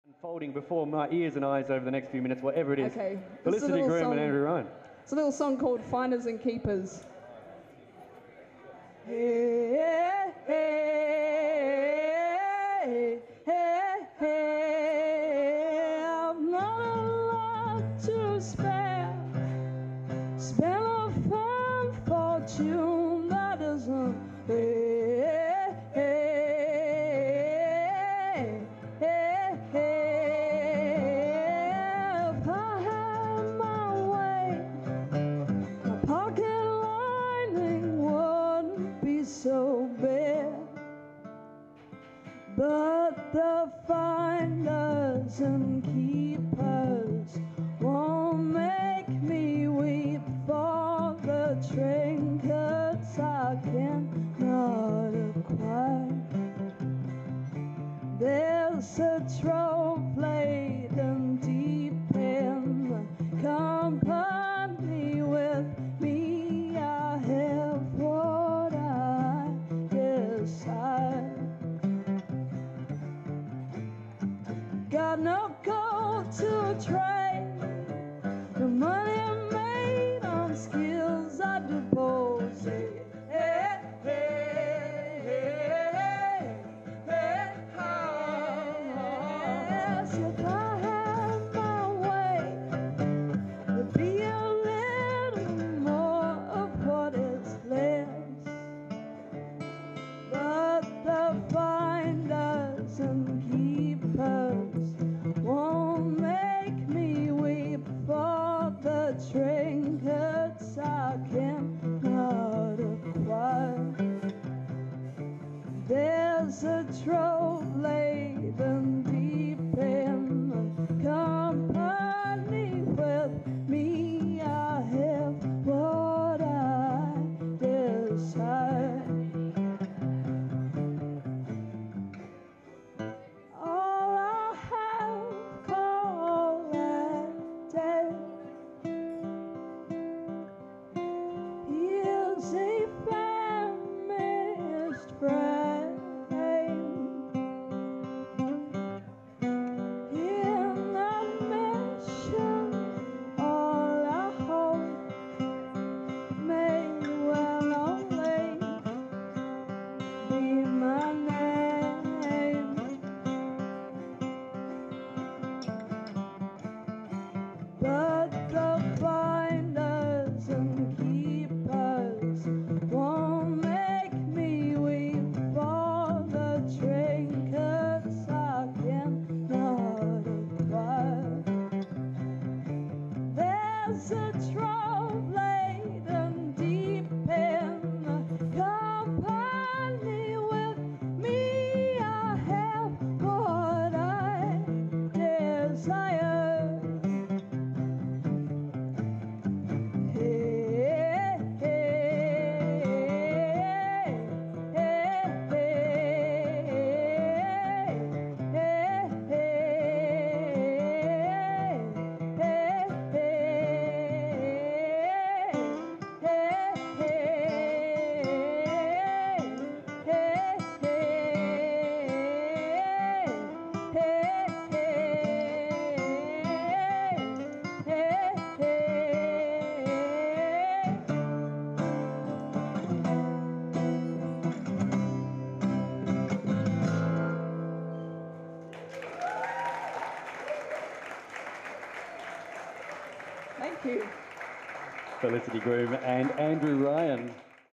RTR FM RADIOTHON // FELICITY GROOM
This year Radiothon opened with an Outside Broadcast from a cafe in the Art Gallery of WA. Breakfast host Peter Barr was joined by smorgasboard of guests.
Felicity Groom and her Autoharp are joined by Andrew Ryan to bring you this tune from her forthcoming album
Distributed by Tubemogul.